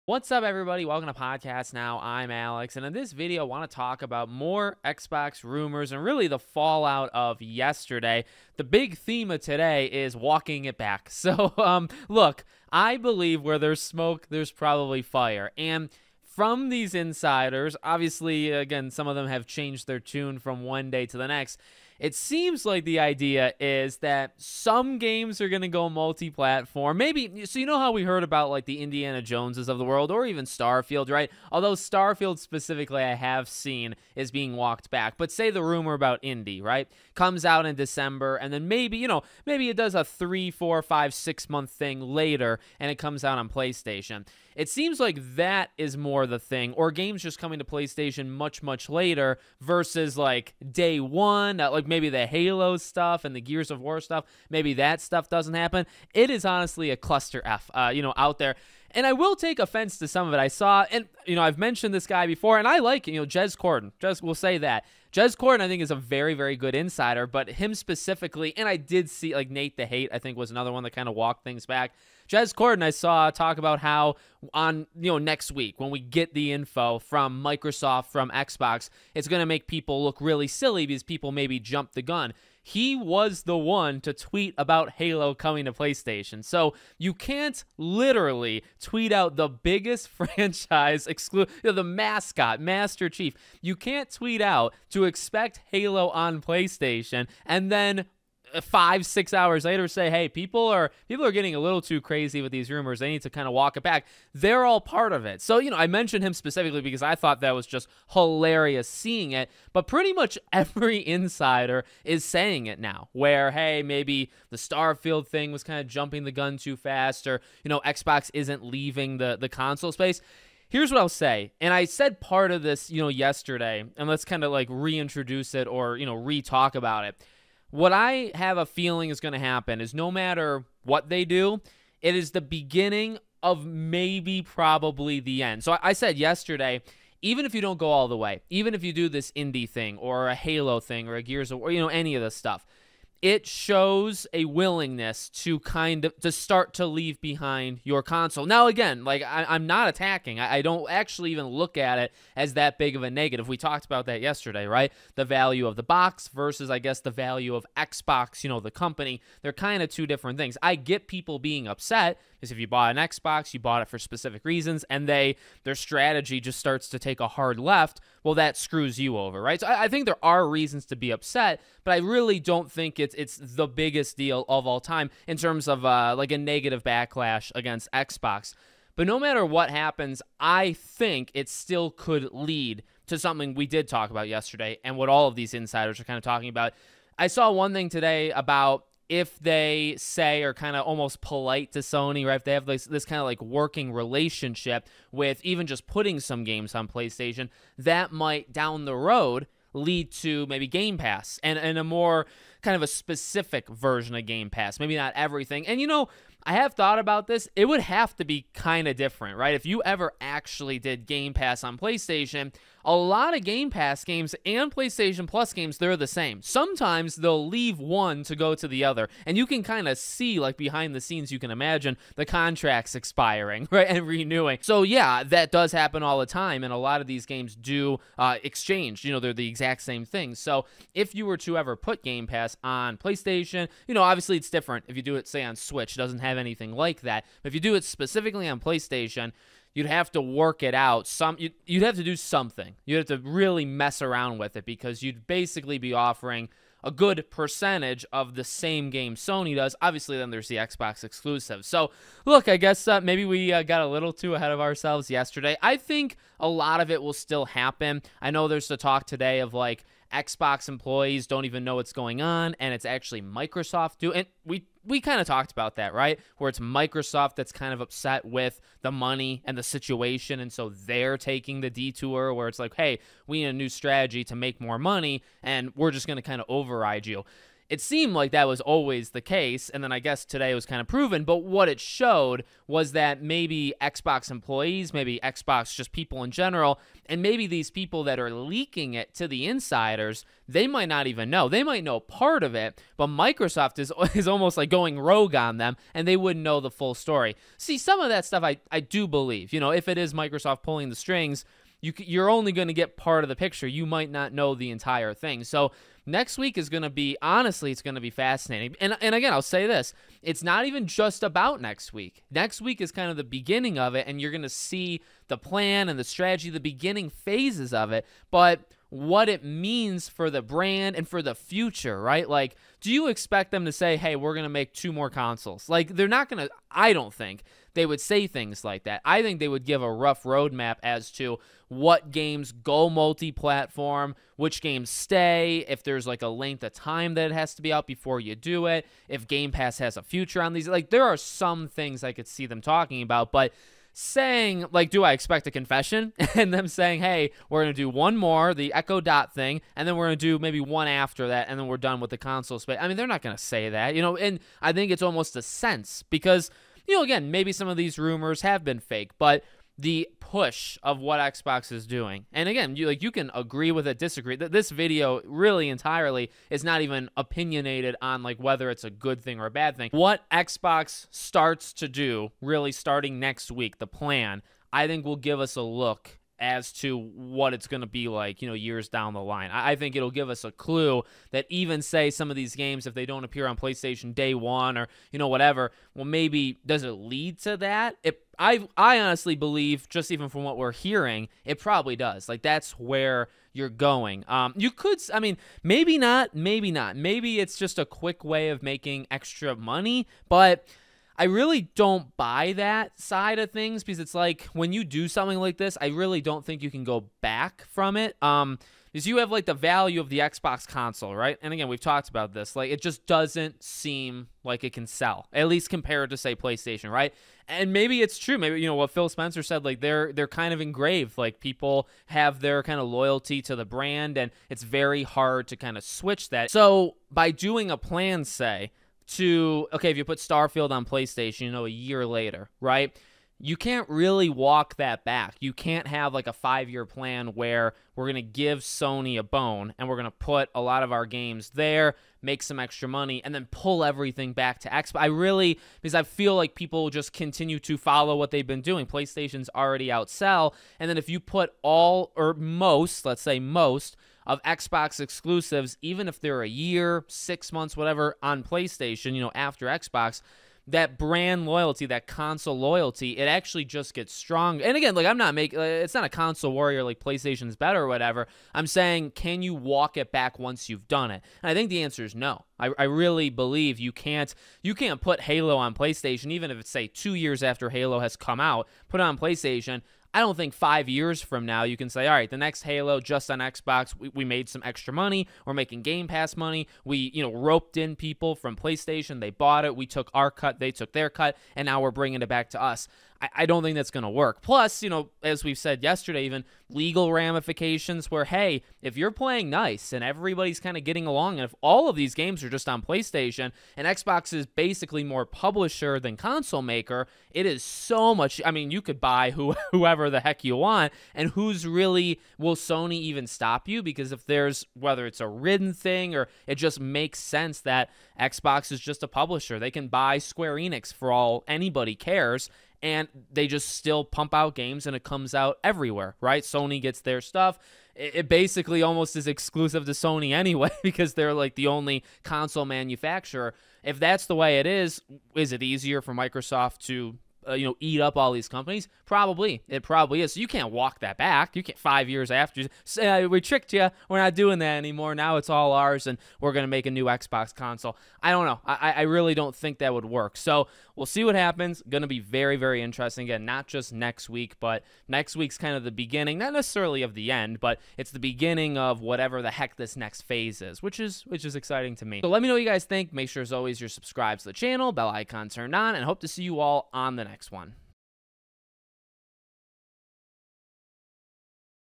So We're Walking Back The Xbox Rumors Now?
Let's talk about those Xbox rumors and them seemingly being walked back? What will happen next week?